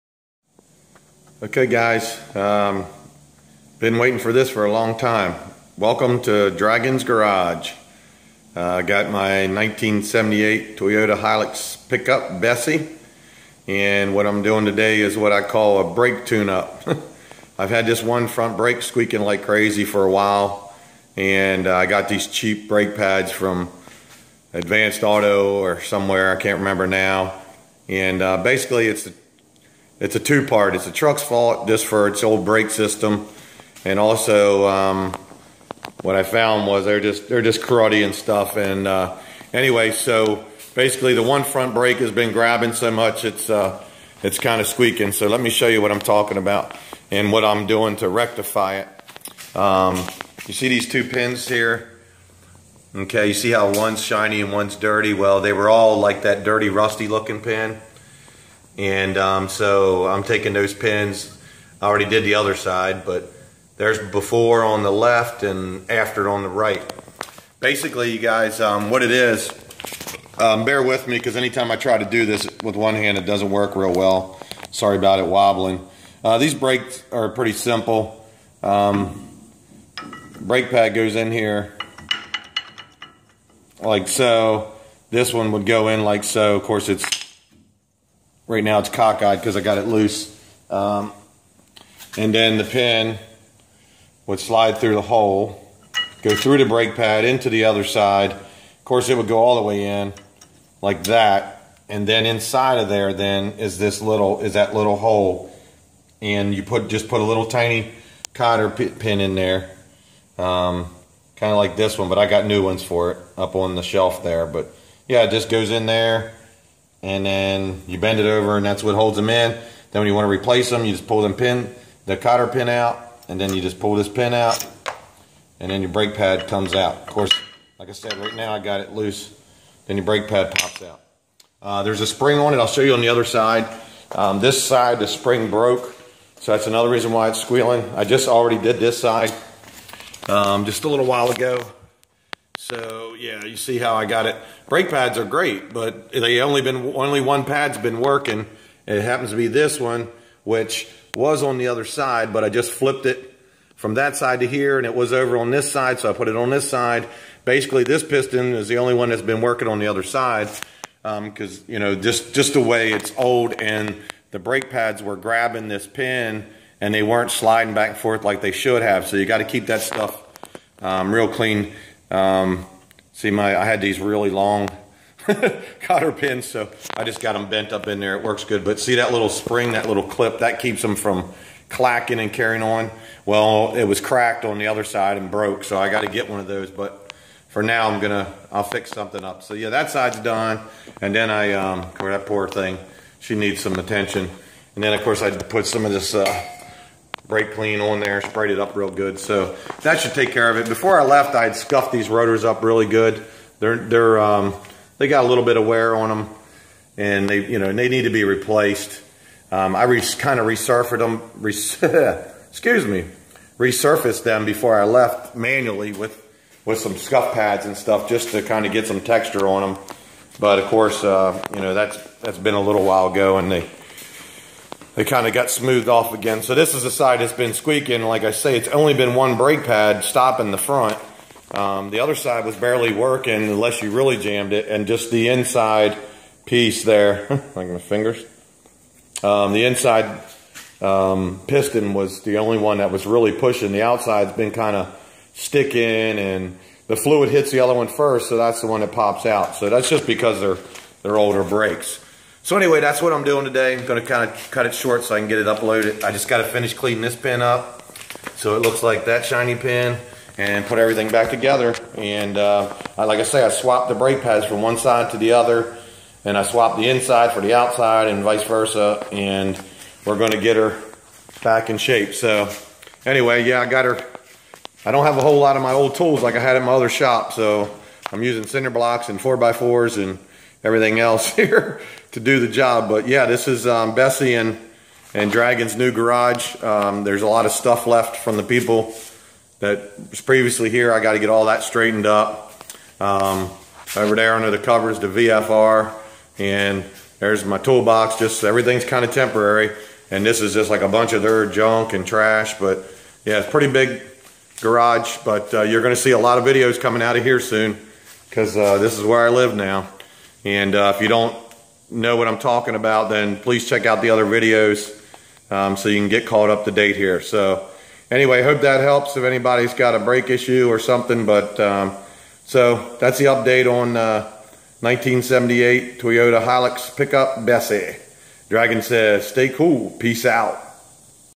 1978 Toyota Hilux Pickup Brake Tune up...fix squealing brake pad.
78 hilux pick up squealing brake pad fix